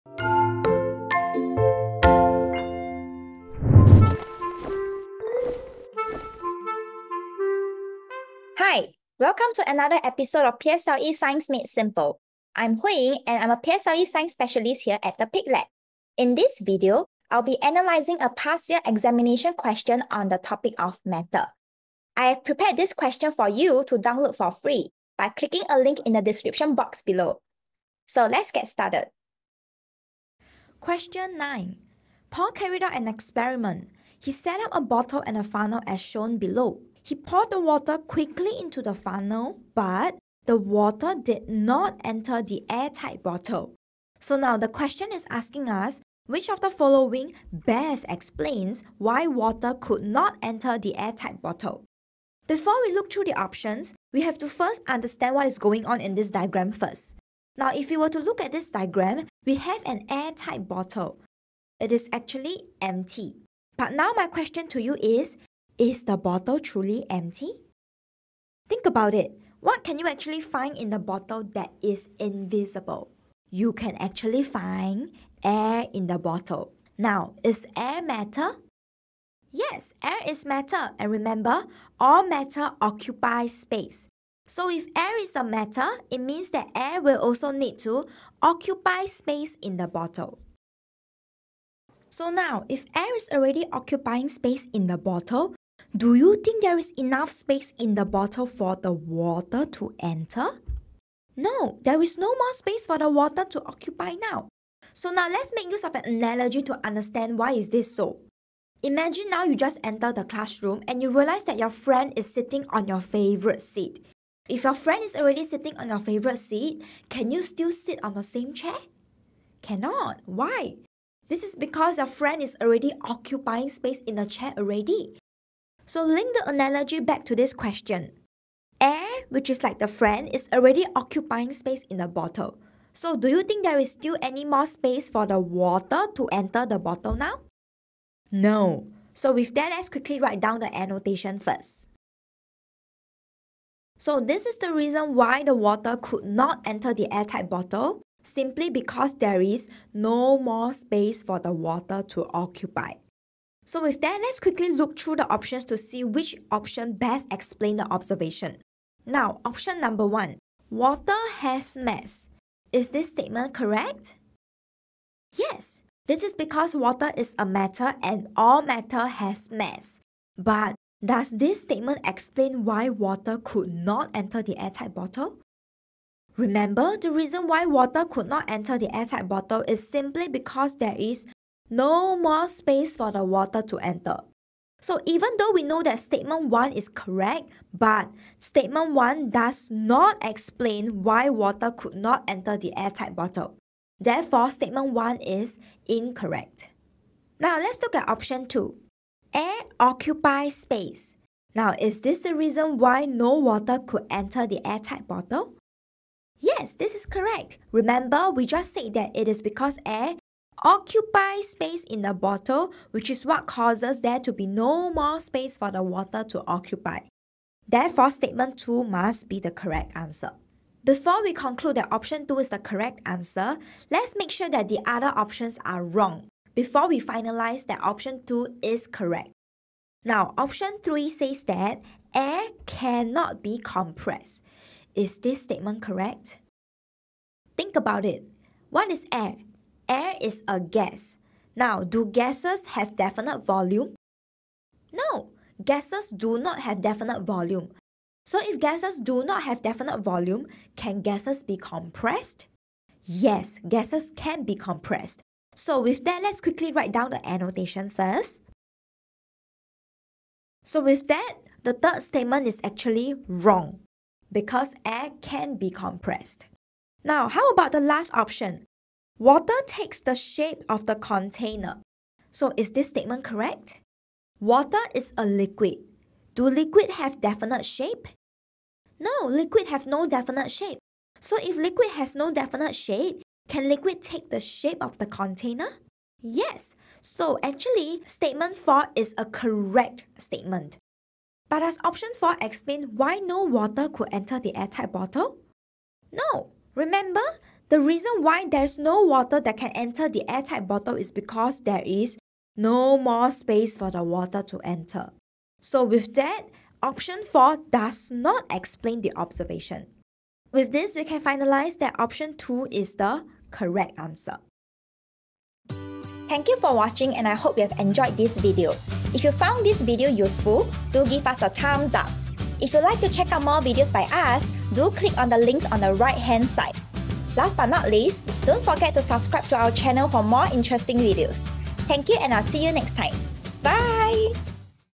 PSLE SCIENCE MADE SIMPLE EP168 | Matter | Why Can’t Water Be Poured Into An Air-Tight Container? 🫙
In this PSLE Science Made Simple episode, Ms. Hui Ying will be analysing a 2019 Primary 4 Science examination question on the topic of Matter from Singapore Chinese Girls' School (SCGS). Watch closely as she shares her approach in tackling this question on Matter!